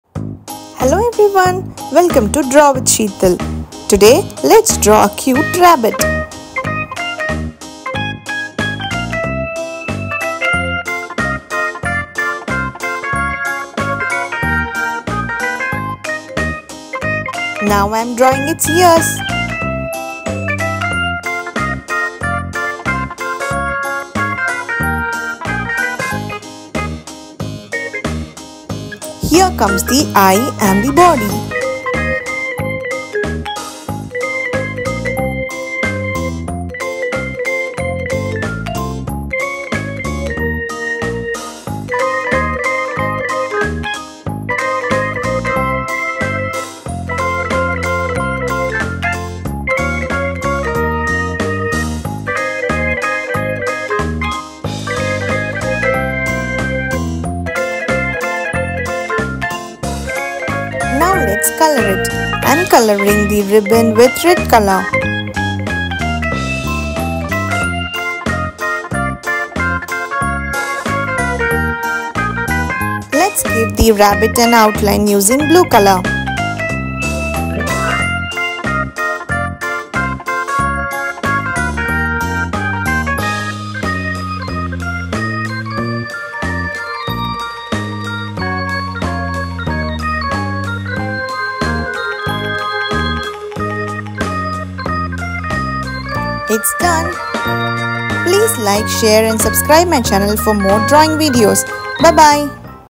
Cute Rabbit with Bow Drawing, Painting and Colouring for kids and toddlers | Easy drawings
In this you can learn how to draw and colour rabbit step by step and follow along my drawing video. Hope you all love this video. More Colourful drawings are yet to come.

With love,
Sheethal ❤️